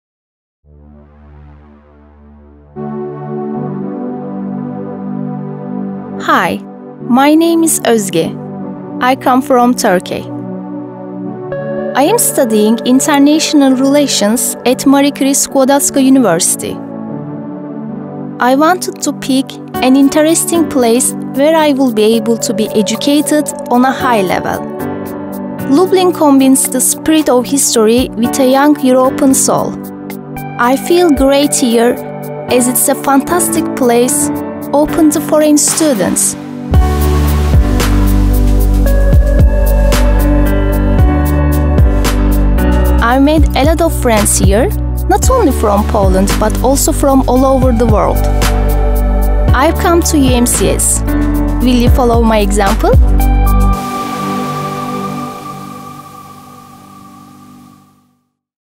UMCS: the students experience
Maria Curie-Sklodowska University in Lublin (UMCS) educates students, who will realize their potential in a certain professional area. The university tries to support its mentees at every stage of their education, but it is not limited solely to the transfer of dry knowledge. It enables them to use it in practice, develop skills and realize passions by participating in various conferences and different scientific societies or meetings with interesting people. It is something more than just the university. This society motivates, expands horizons and shows a number of opportunities connected with development.